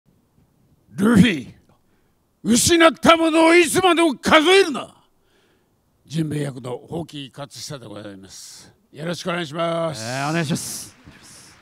【ワンピース】ジンベエのセリフを宝亀克寿が生披露！
宝亀克寿（ジンベエ役）が1月28日、『ONE PIECE FILM RED』フィナーレ大宴に登場した。